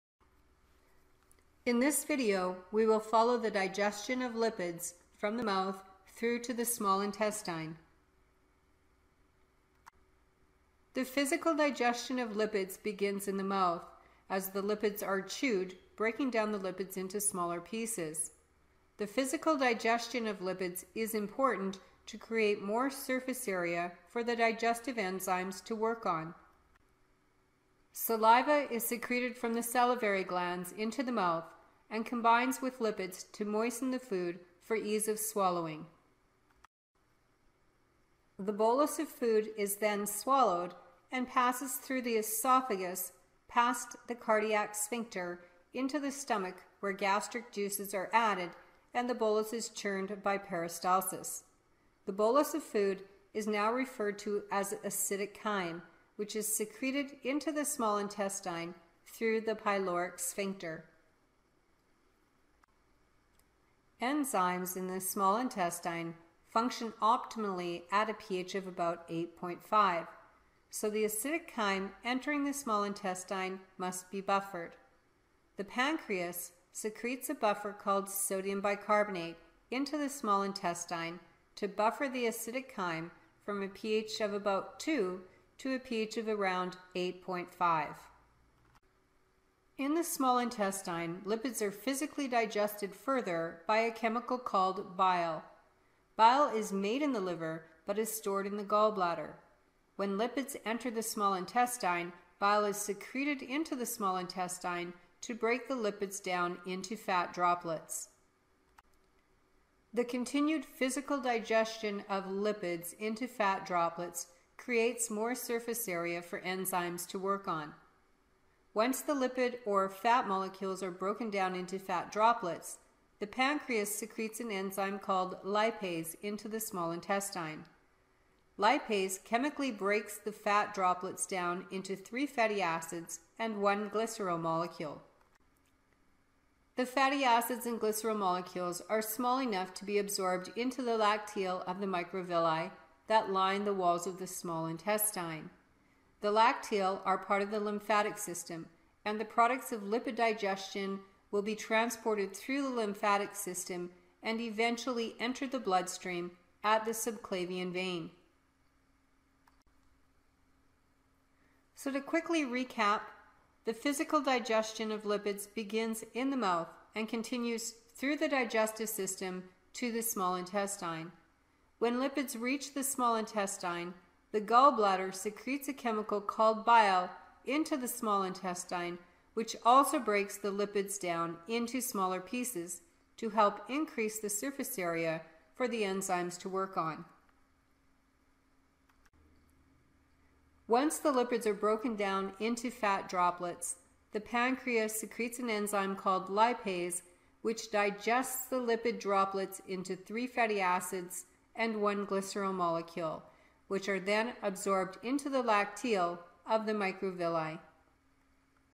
WCLN - Lipid Digestion - Biology
Describes the digestion of lipids from the mouth to the small intestine.